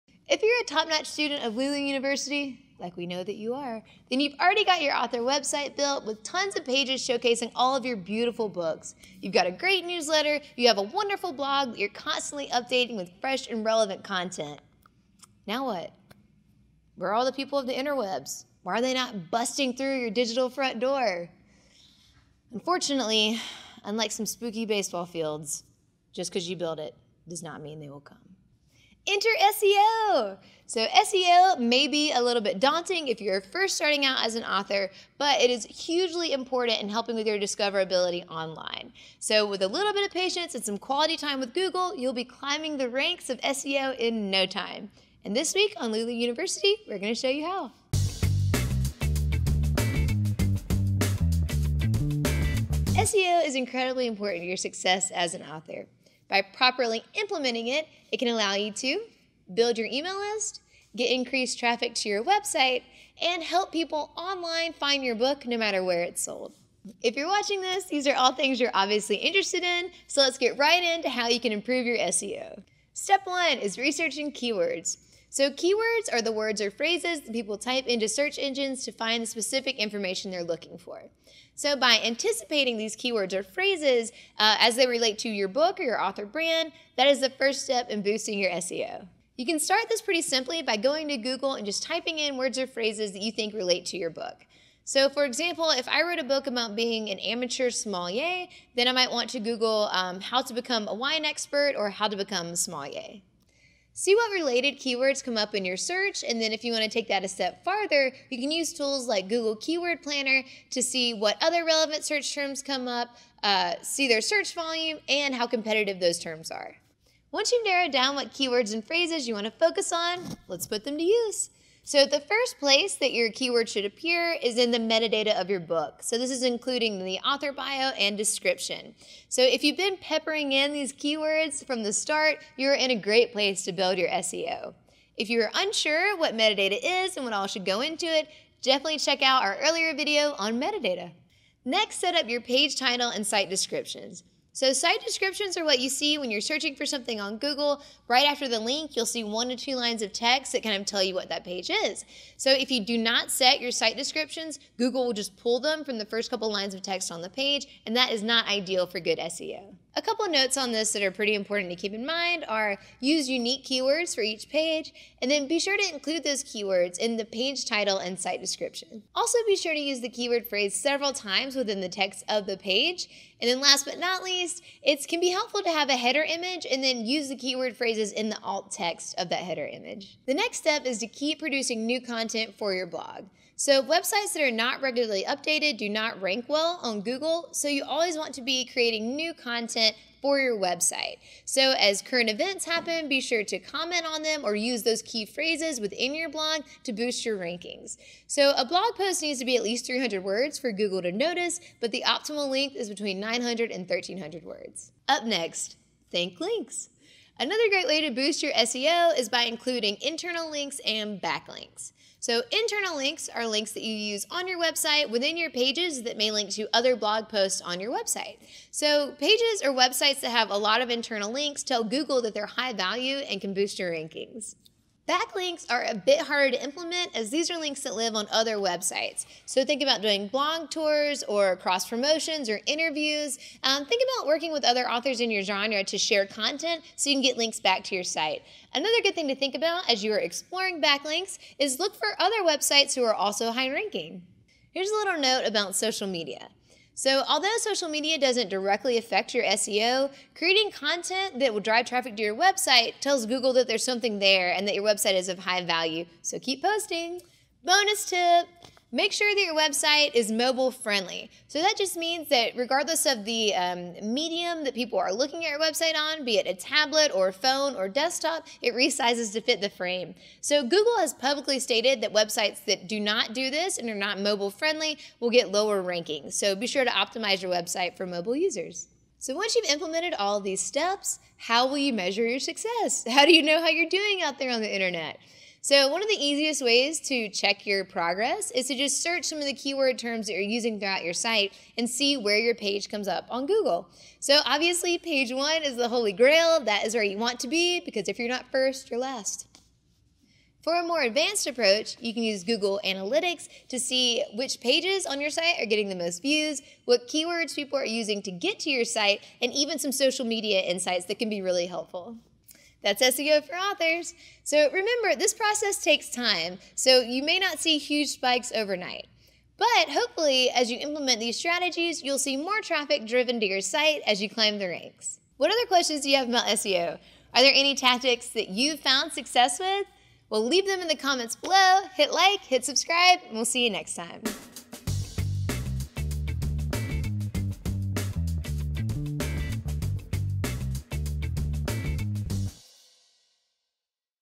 SEO for Authors - How to Optimize Your Website for a Better Ranking
SEO marketing is one of the most important ways to expand your reach as an author. In this episode of Lulu University, we'll talk about how to optimize your author website to improve your search rankings and boost visibility using proven SEO strategies!

Lulu.com is the original self-publisher, offering print on demand services to meet your needs.

Tell Your Story. Share Your Knowledge. The World is Waiting.